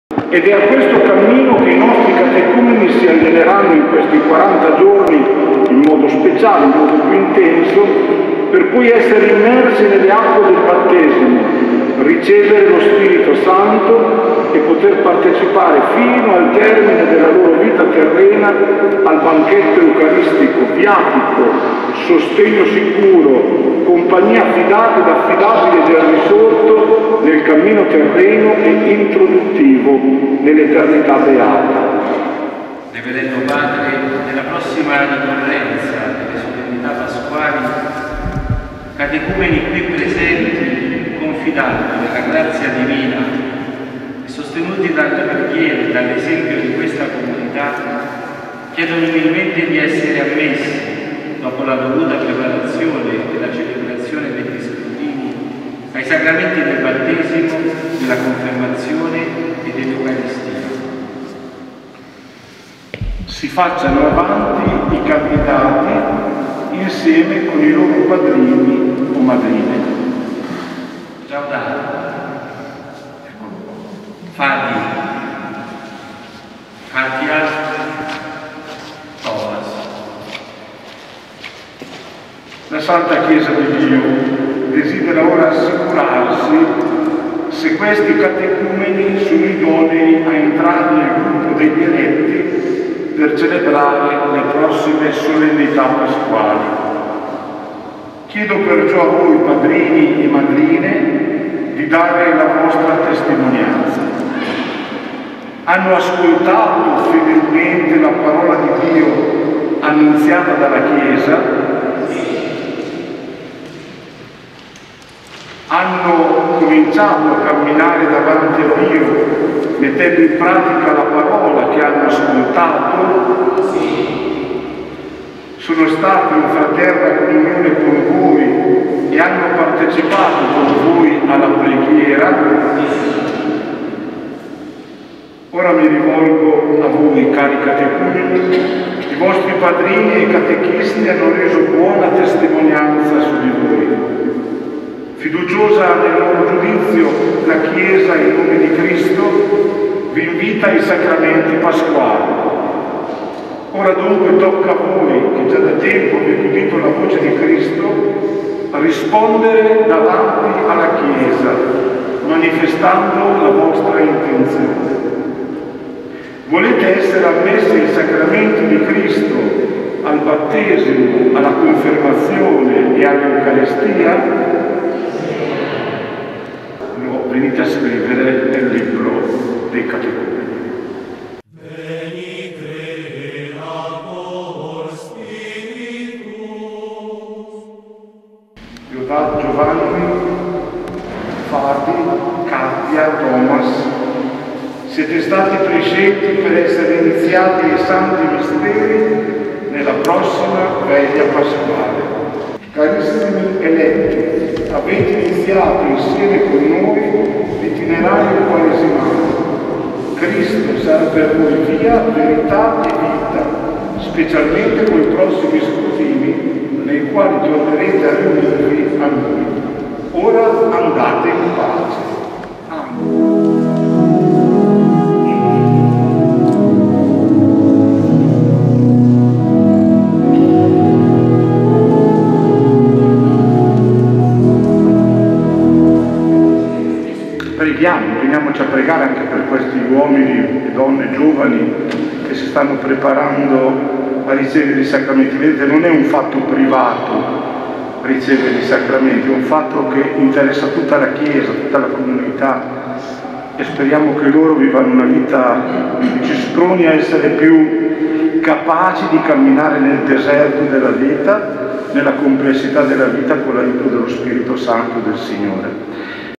Catecumeni verso il battesimo nella Veglia Pasquale
Tivoli (Rm): quattro catecumeni, Jaoudat, Fadi, Katia e Thomas, durante la prima messa quaresimale nella cattedrale di San Lorenzo Martire, officiata dal vescovo di Tivoli e Palestrina, monsignor Mauro Parmeggiani, sono stati ammessi ed iscritti nel registro dei Catecumeni, per ricevere i sacramenti del Battesimo, della Comunione e della Cresima dopo un periodo di discernimento e formazione, seguiti dai propri catechisti.